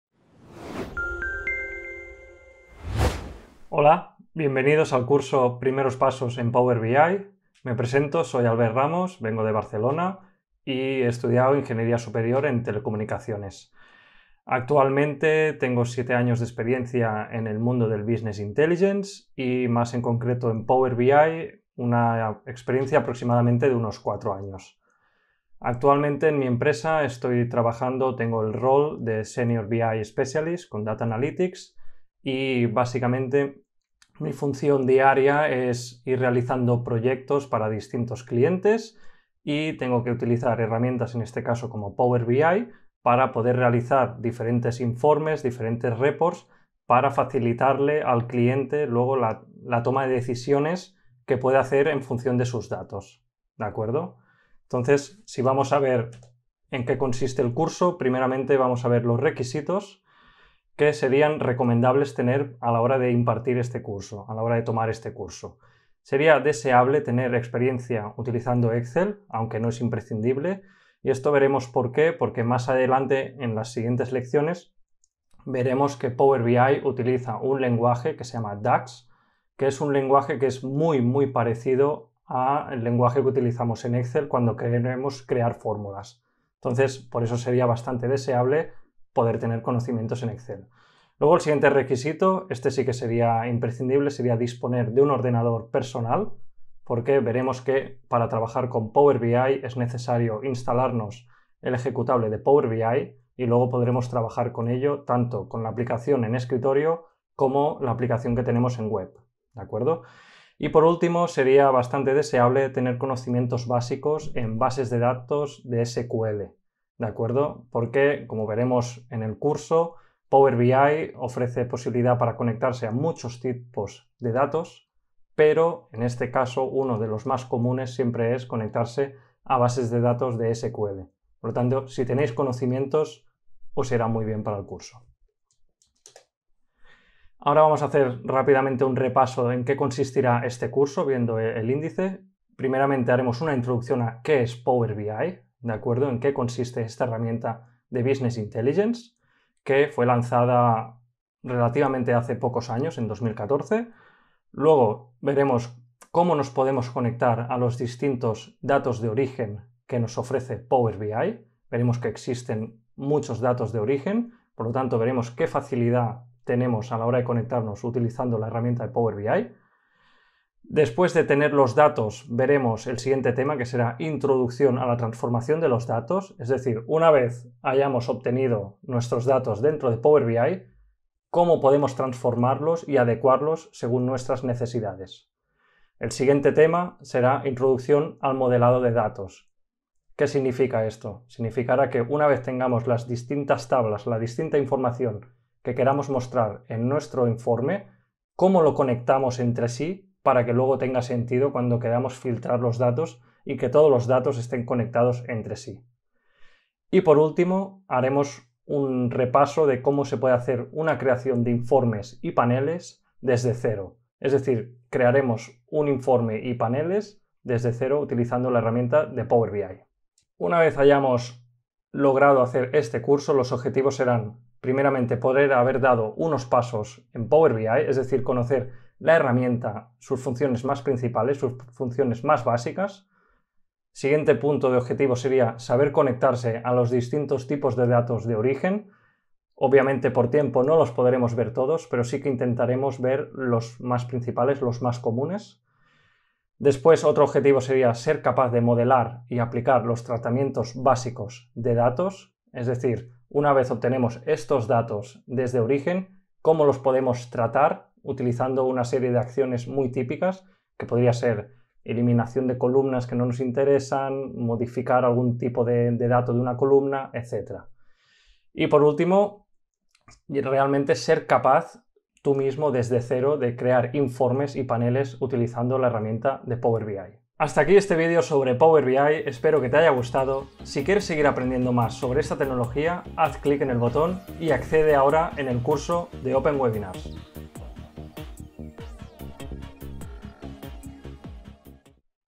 Curso de Power BI: Introducción
Con este curso de introducción a Power BI comenzarás a aprender a utilizar una de las herramientas más potentes del mercado para el uso y explotación de los datos, con la que podrás crear todo tipo de informes y diseños.

- Conectarse a distintos tipos de datos de origen
- Modelado y tratamiento de datos
- Creación de informes y paneles dinámicos